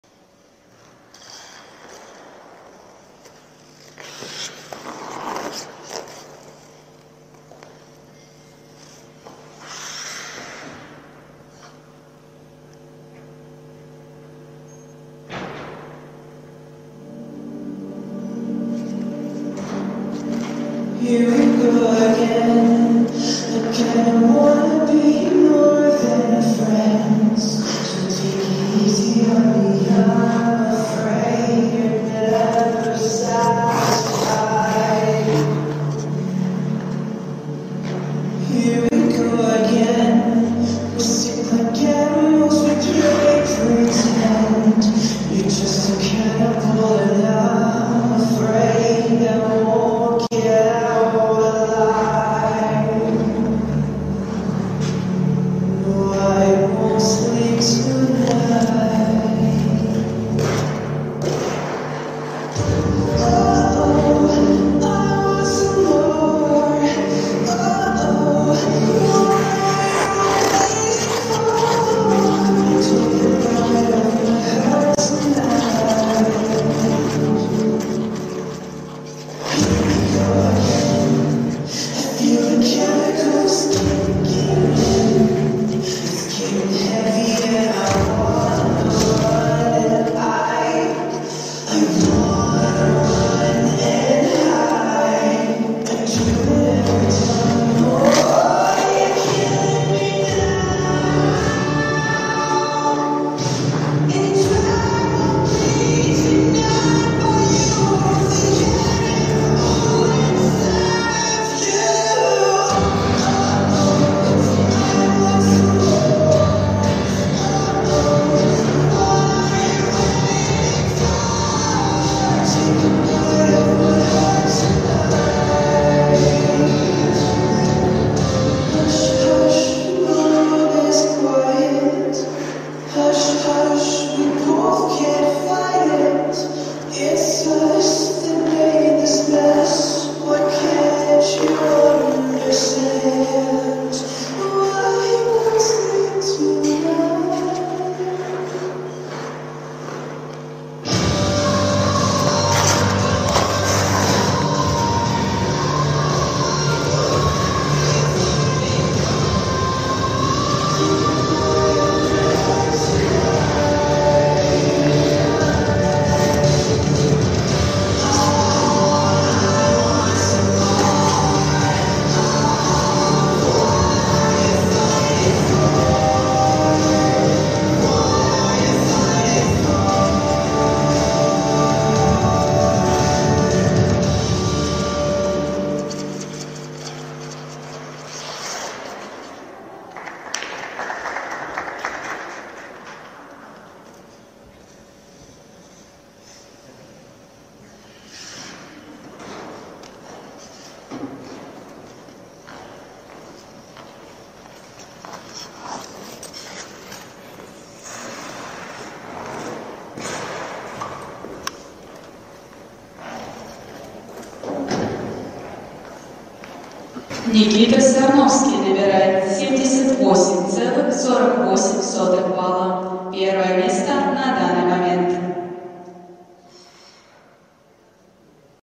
Никита Сарновский (КП) - Первенство Москвы (старший возраст)
16 лет, АФК Ангелы Плющенко, тренер Плющенко
Оценка: 78.48